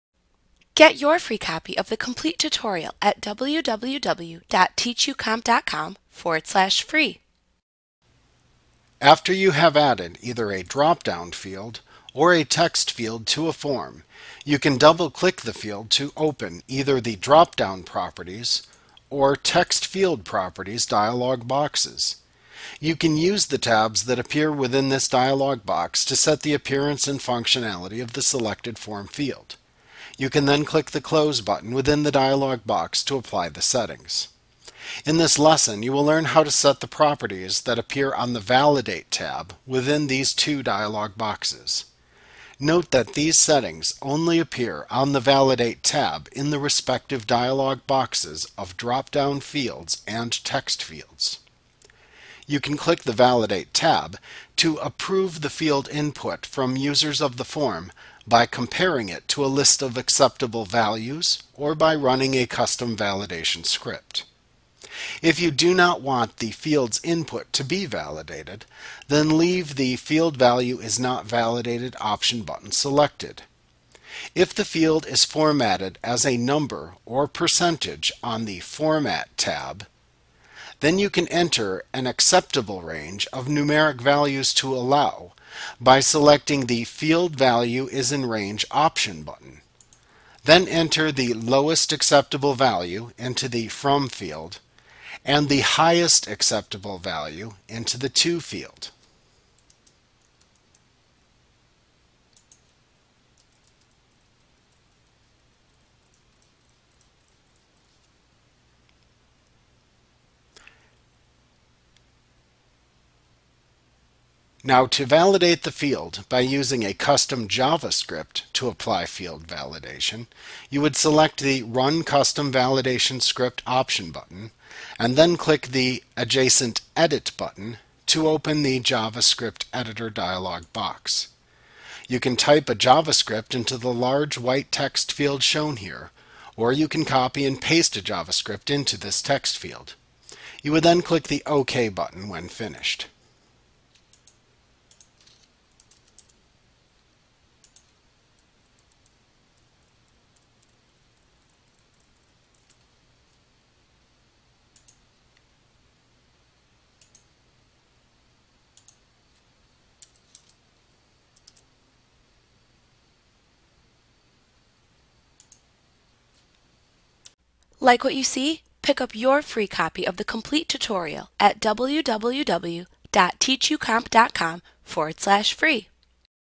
Validate Properties of Form Fields - Adobe Acrobat XI Training Tutorial Course
Learn about Validate Properties of Form Fields in Adobe Acrobat XI Visit us today!